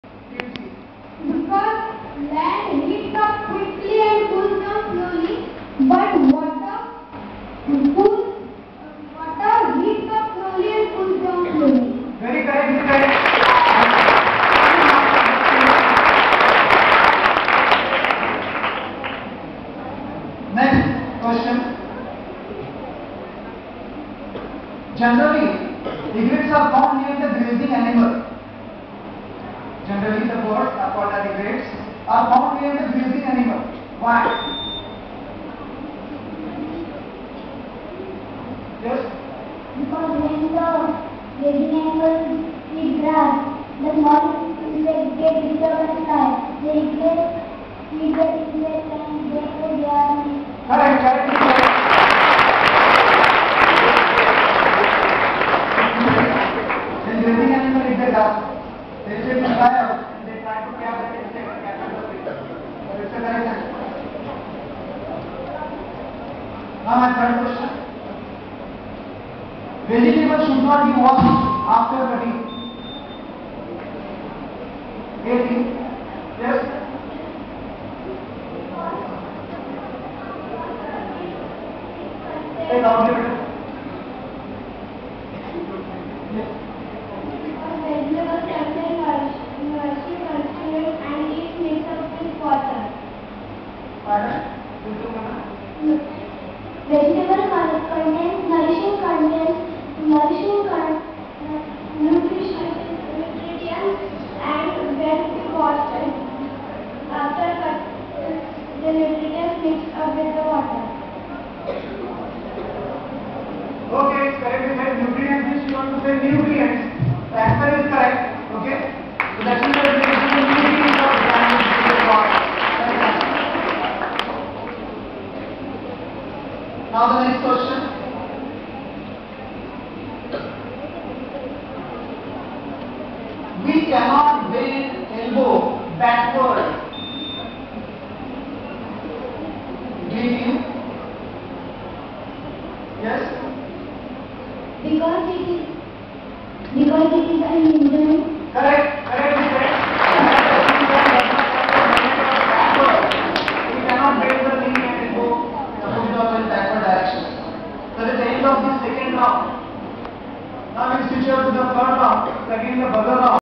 Science Meet 2014 Science Quiz round
Science Quiz round Conducted by Resp.Rajeshji Jodh sir in Science Meet 2014 Selected Groups on stage from Schools - School of Scholars (main branch), Prabhat Kids, Nishu Nursery,  School of Scholars (birla colany) akola